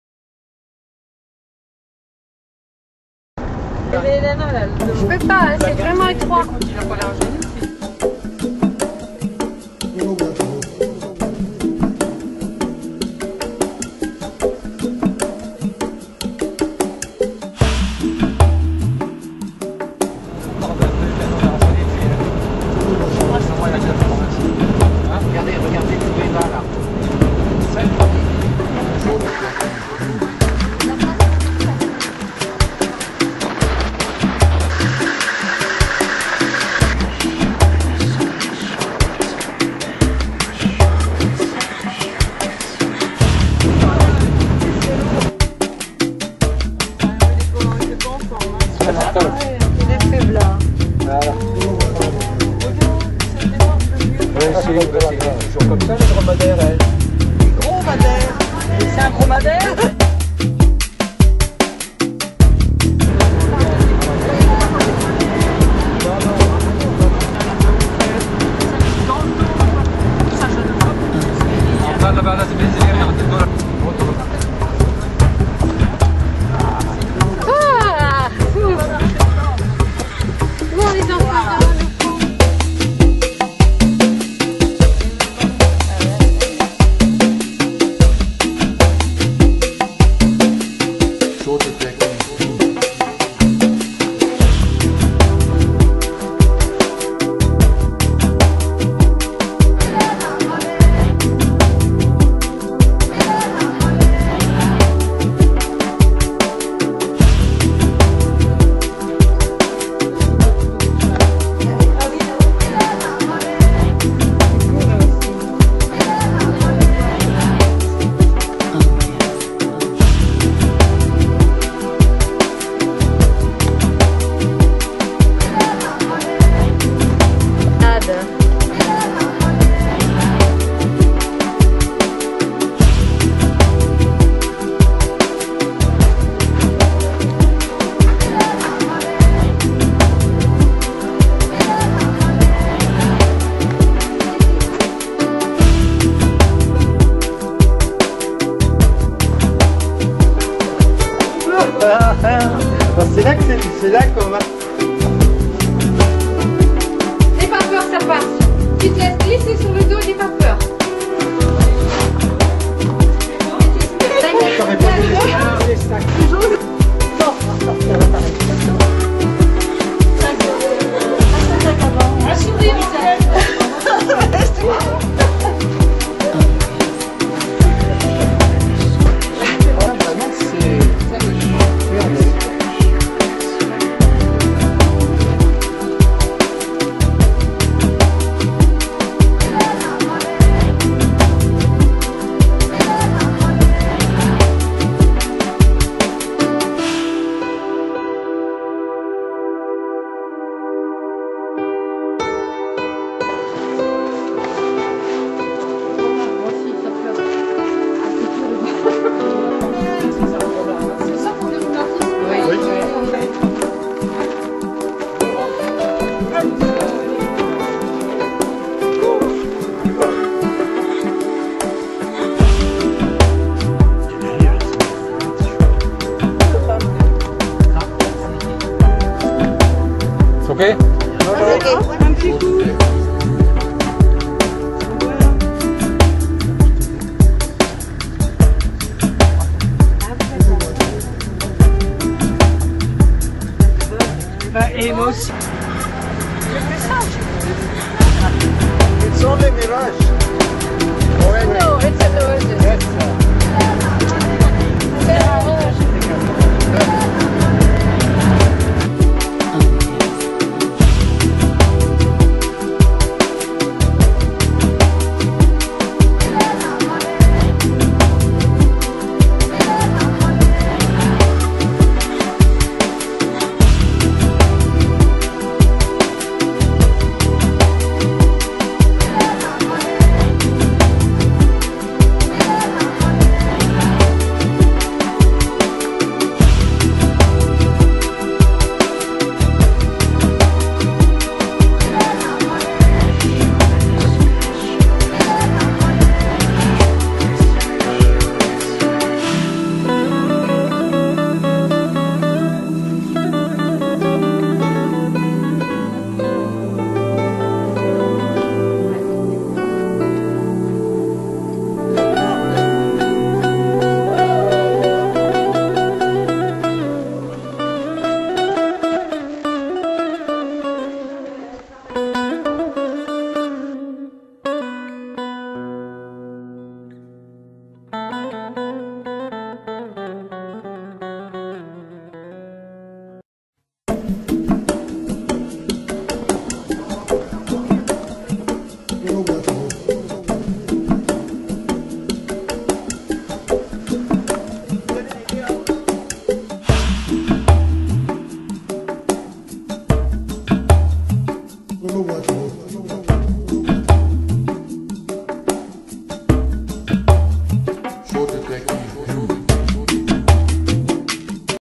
white canyon'
A bird fight with himself in Mirror of of our Jeep ,
Baby Camel with his mother ,
Time for the Lizards to go out .